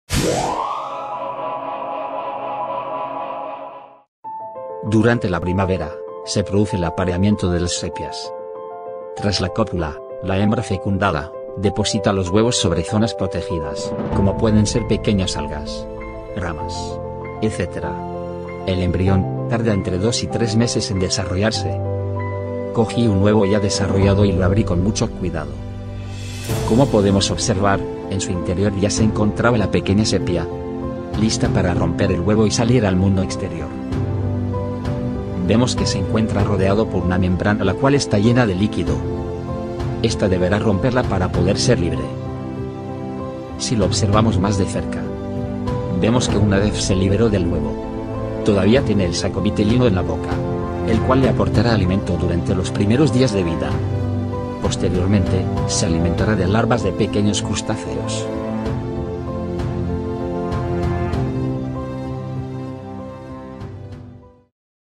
Sepia. Observo el nacimiento de una jibia/sepia🦑
Así es una jibia recién nacida.